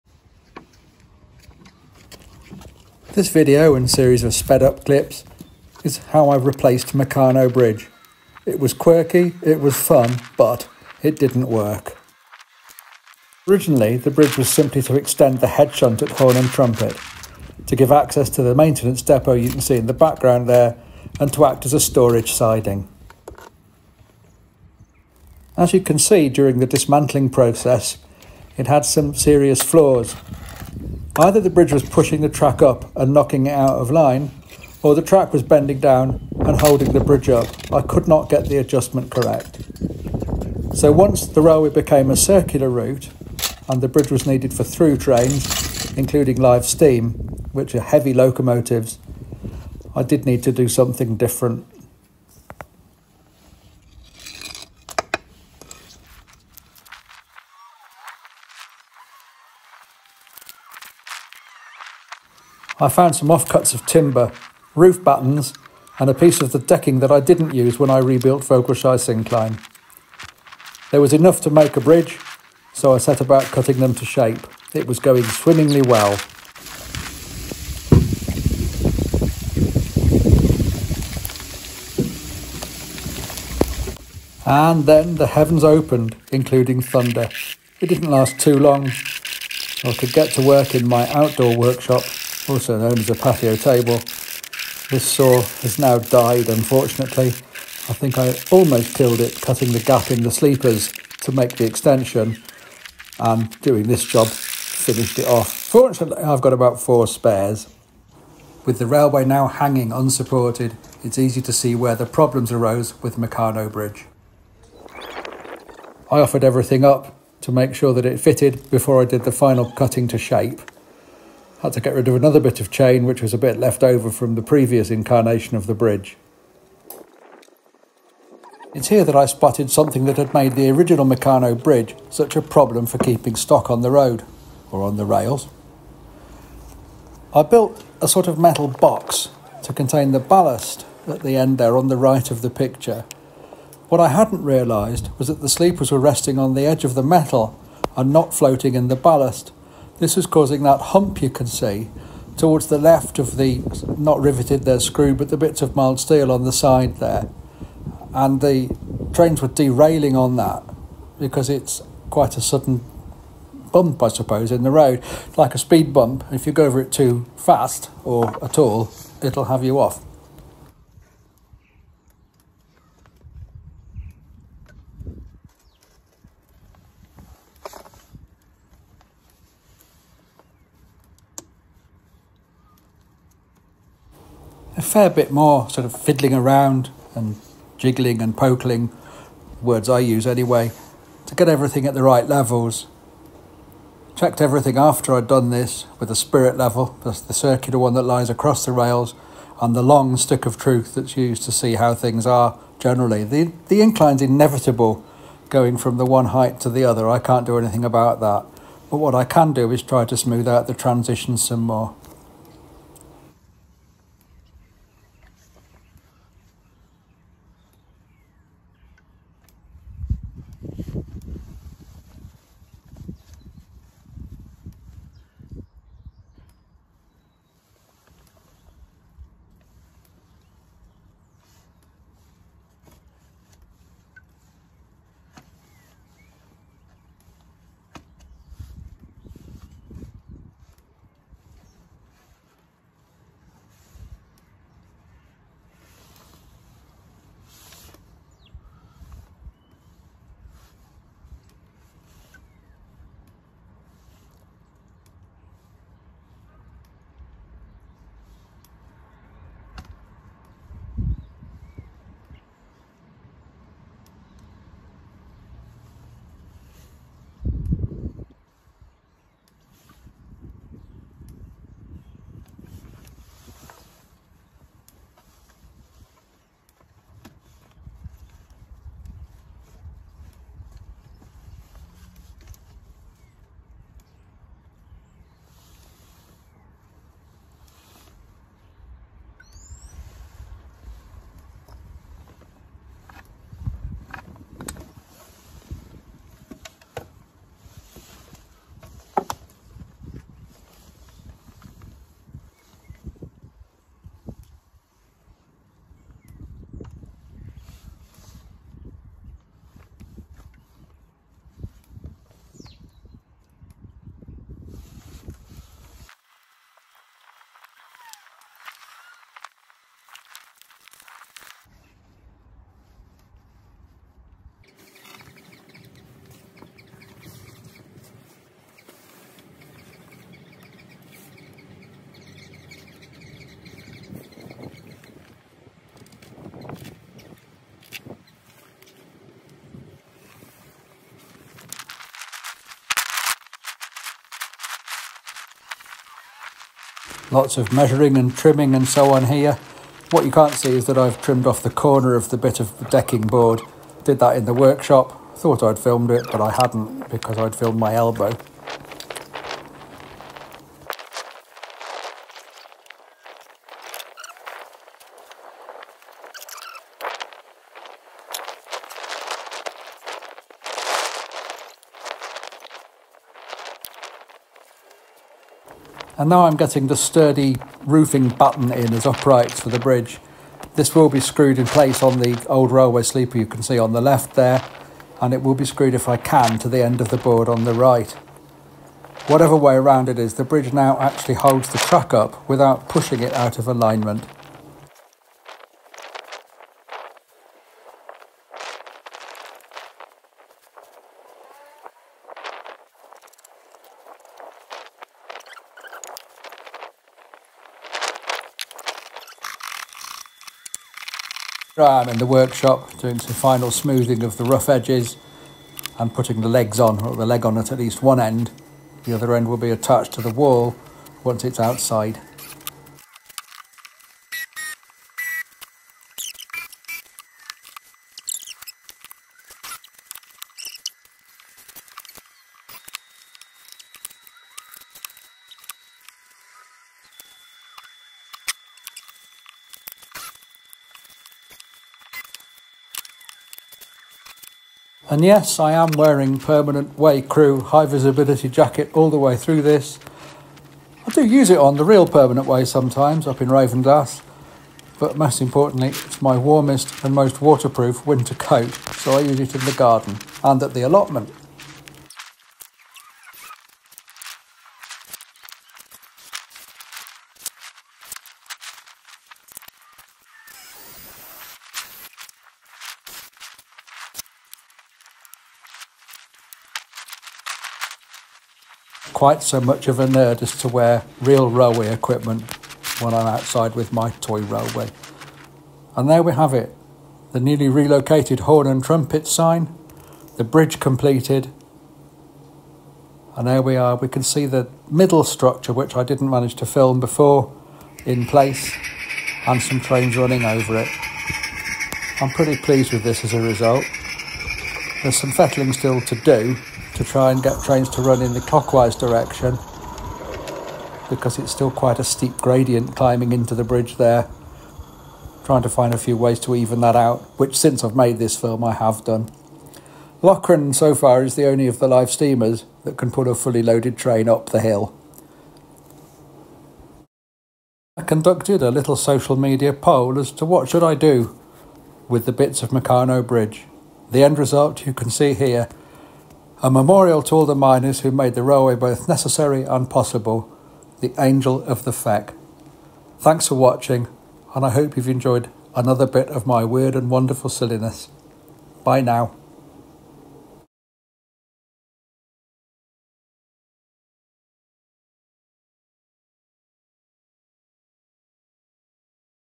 P-Way Wednesday (workshopwednesday) - Replacing Meccano Bridge with McGonagall Bridge
I have made more changes since filming this, both aesthetic and functional. The ‘castle’ that acts as a rain cover for the point motor switchgear now sits square and I’ve trimmed the ends off a few sleepers so that the track at the southern end of Hornundtrompetebahnhof now actually floats on ballast rather than looking like it does.
The exposed cabling has also been tidied up.